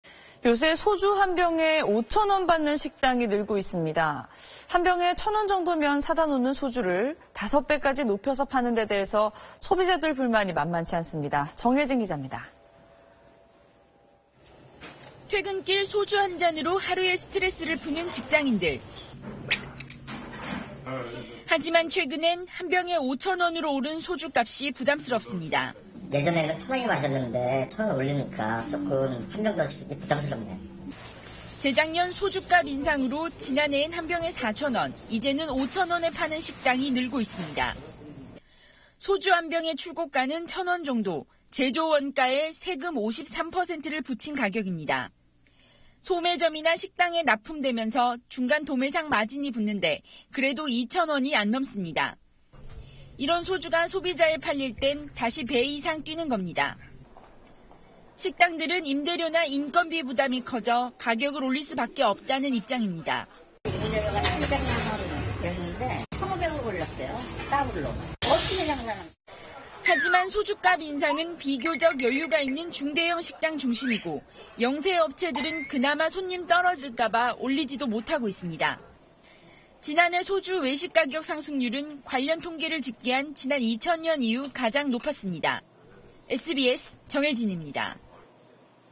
소주 출고가는 천 원, 식당은 5천 원…차이 나는 까닭 / SBS
요새 소주 한 병에 5천 원 받는 식당이 늘고 있습니다. 한 병에 천 원 정도면 사다놓는 소주를 다섯 배까지 높여서 파는 데 대해서, 소비자들 불만이 만만치 않습니다.